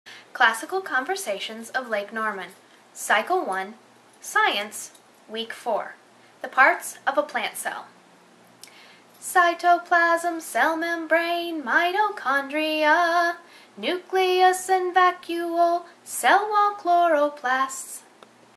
Classical Conversations of Lake Norman - Parts of a Plant Cell
Classical Conversations of LKN Cycle 1 Science Week 4

The parts of a plant cell:

Cytoplasm
Cell Membrane
Mitochondria
Nuclueus
Vacuole
Cell Wall
Chloroplasts